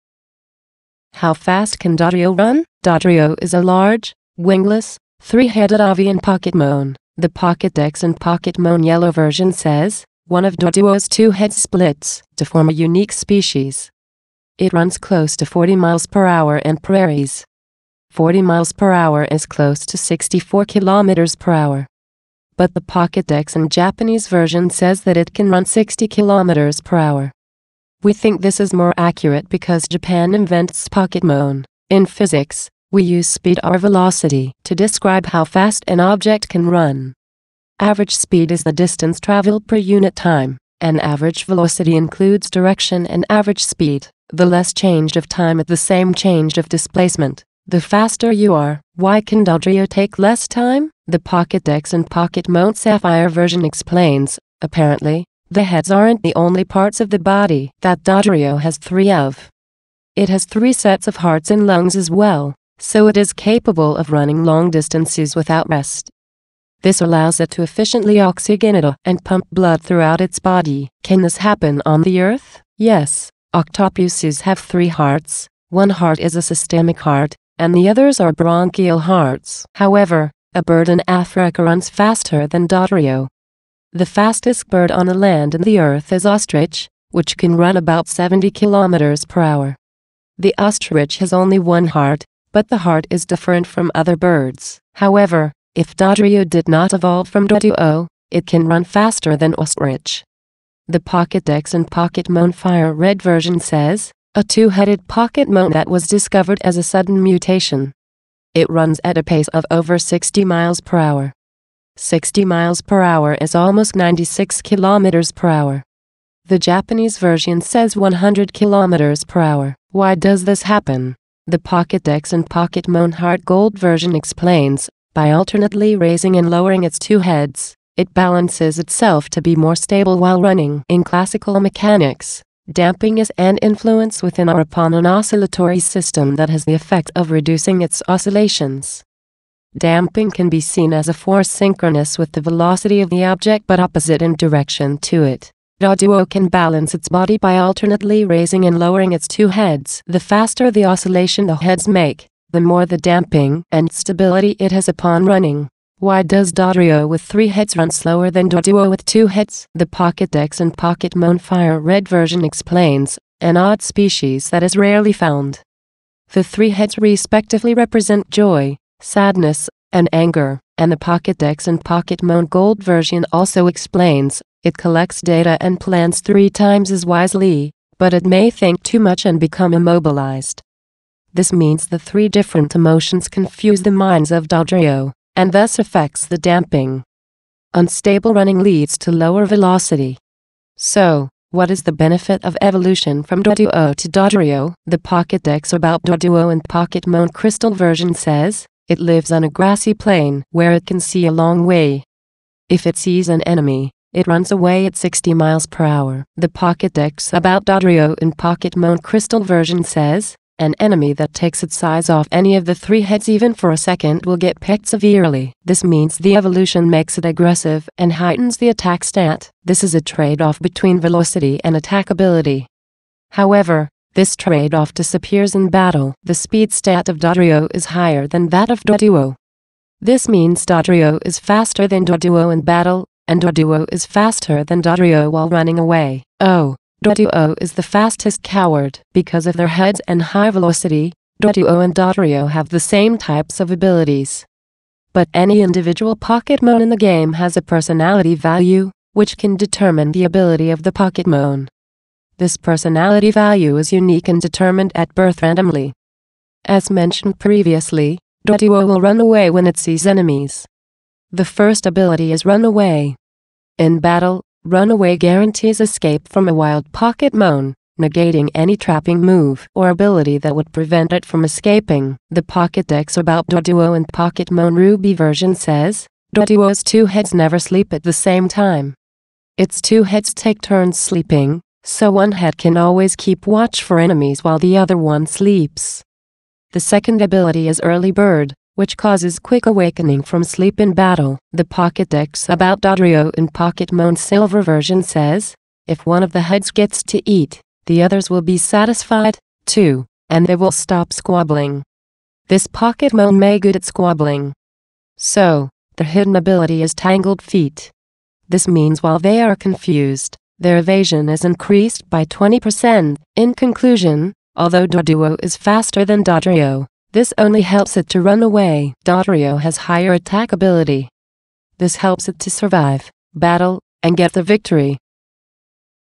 Pokemon in Science: How fast can Dodrio run?
If you do not like the sound, you can mute it by a button.

How fast can Dodrio run? Dodrio is a large, wingless, three-headed avian Pokemon. The Pokedex in Pokemon Yellow Version says:" One of Doduo's 2 heads splits to form a unique species. It runs close to 40 MPH in prairies. " 40 MPH is close to 64 kilometers per hour. But the Pokedex in Japanese version says that it can run 60 kilometers per hour. We think this is more accurate because Japan invents Pokemon.

In physics, we use speed or velocity to describe how fast an object can run. Average speed is the distance traveled per unit time, and average velocity includes direction and average speed. The less change of time at the same change of displacement, the faster you are.

Why can Dodrio take less time? The Pokedex in Pokemon Sapphire Version explains:" Apparently, the heads aren't the only parts of the body that Dodrio has three of. It has three sets of hearts and lungs as well, so it is capable of running long distances without rest. " This allows it to efficiently oxygenate and pump blood throughout its body.

Can this happen on the Earth? Yes, Octopuses have three hearts, one heart is a systemic heart, and the others are branchial hearts. However, a bird in Africa runs faster than Dodrio. The fastest bird on the land in the Earth is ostrich, which can run about 70 kilometers per hour. The ostrich has only one heart, but the heart is differnt from other birds'.

However, if Dodrio did not evolve from Doduo, it can run faster than ostrich. The Pokedex in Pokemon FireRed Version says:"A two-headed Pokemon that was discovered as a sudden mutation. It runs at a pace of over 60 miles per hour. " 60 miles per hour is almost 96 kilometers per hour. The Japanese version says 100 kilometers per hour.

Why does this happen? The Pokedex in Pokemon HeartGold Version explains:" By alternately raising and lowering its two heads, it balances itself to be more stable while running. " In classical mechanics, damping is an influence within or upon an oscillatory system that has the effect of reducing its oscillations. Damping can be seen as a force synchronous with the velocity of the object but opposite in direction to it. Doduo can balance its body by alternately raising and lowering its two heads. The faster the oscillation the heads make, the more the damping and stability it has upon running.

Why does Dodrio with 3 heads run slower than Doduo with 2 heads? The Pokedex in Pokemon FireRed Version explains:" An odd species that is rarely found. The three heads respectively represent joy, sadness, and anger.” And the Pokedex in Pokemon Gold Version also explains:" It collects data and plans three times as wisely, but it may think too much and become immobilized.” This means the 3 different emotions confuse the minds of Dodrio, and thus affects the damping. Unstable running leads to lower velocity.

So, what is the benefit of evolution from Doduo to Dodrio? The Pokedex about Doduo in Pokemon Crystal Version says:” It lives on a grassy plain where it can see a long way. If it sees an enemy, it runs away at 60 mph.” The Pokedex about Dodrio in Pokemon Crystal Version says:” An enemy that takes its eyes off any of the three heads—even for a second—will get pecked severely.” This means the evolution makes it aggressive and heightens the Attack stat. This is a trade-off between velocity and attack ability. However, this trade-off disappears in battle. The Speed stat of Dodrio is higher than that of Doduo. This means Dodrio is faster than Doduo in battle, and Doduo is faster than Dodrio while running away. Oh, Doduo is the fastest coward.

Because of their heads and high velocity, Doduo and Dodrio have the same types of Abilities. But any individual Pokemon in the game has a Personality Value, which can determine the Ability of the Pokemon. This Personality Value is unique and determined at birth randomly.

As mentioned previously, Doduo will run away when it sees enemies. The First Ability is Run Away. In battle, Run Away guarantees escape from a wild Pokemon, negating any trapping move or Ability that would prevent it from escaping.

The Pokedex about Doduo in Pokemon Ruby Version says:” Doduo's two heads never sleep at the same time. Its two heads take turns sleeping, so one head can always keep watch for enemies while the other one sleeps. "The Second Ability is Early Bird, which causes quick awakening from sleep in battle.

The Pokedex about Dodrio in Pokemon Silver Version says: " If one of the heads gets to eat, the others will be satisfied, too, and they will stop squabbling. " This Pokemon may good at squabbling. So, the Hidden Ability is Tangled Feet. This means while they are confused, their evasion is increased by 20%,

In conclusion, although Doduo is faster than Dodrio, this only helps it to run away. Dodrio has higher attack ability. This helps it to survive, battle, and get the victory.